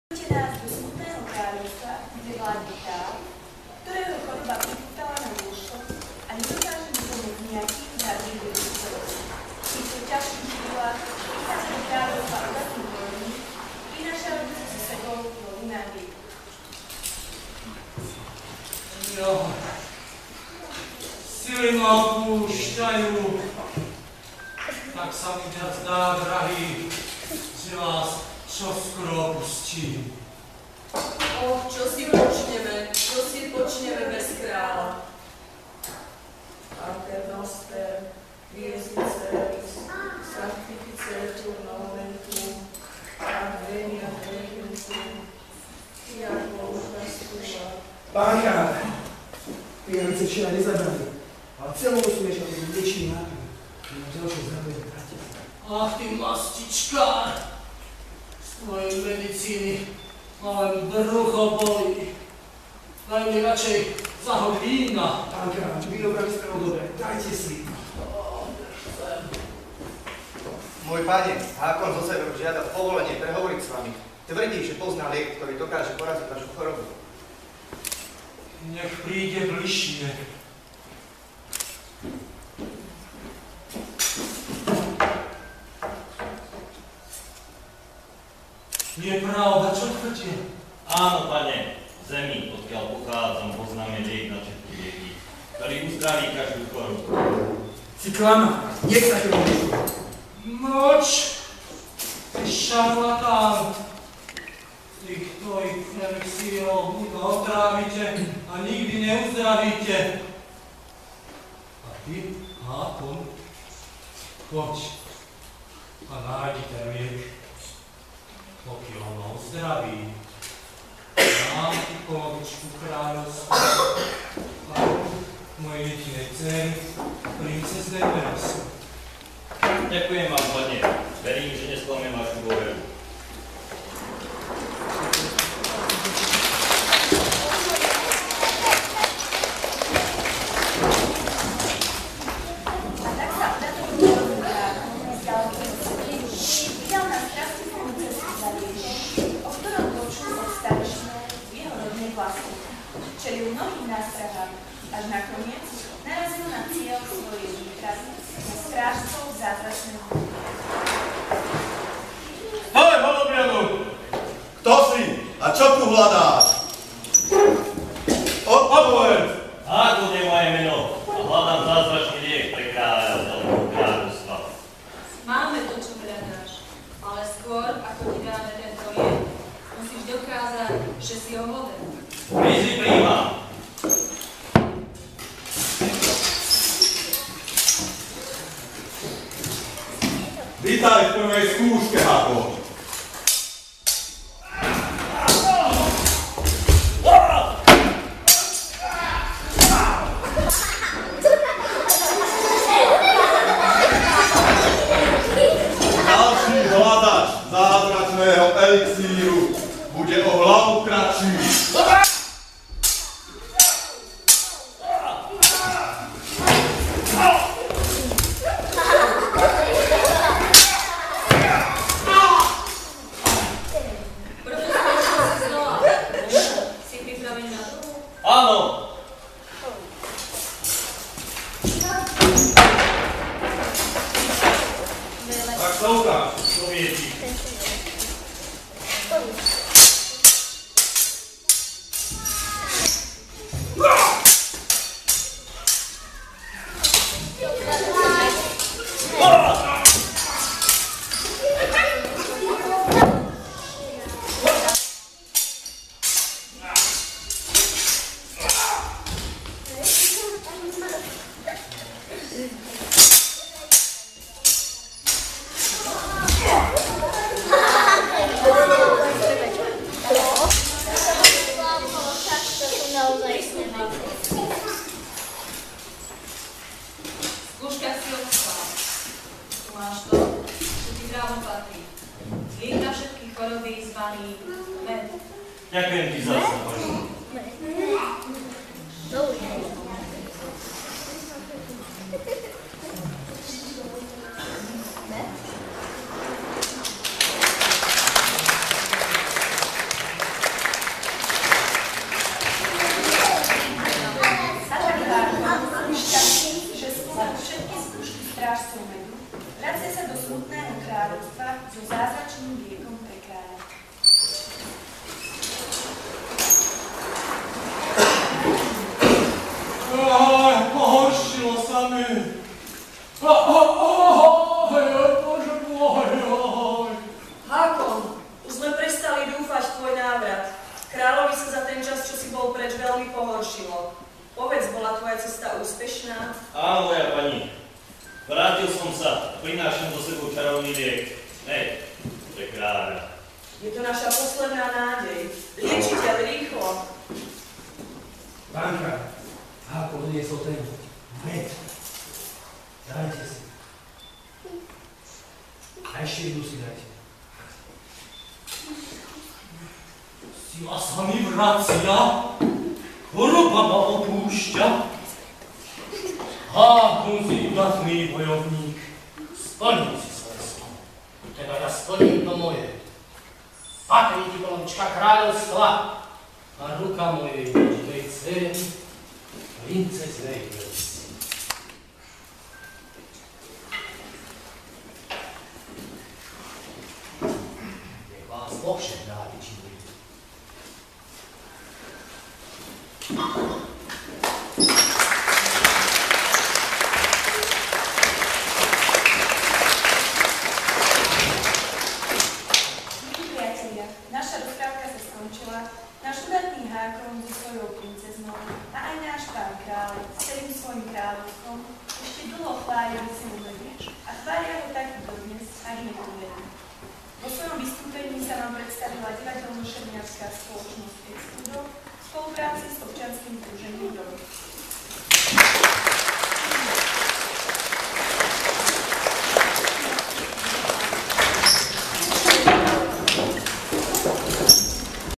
DNI MEDU Stankovany
Rozprávka pre deti o Čarovnom MEDe
Fairy tale about Magical honey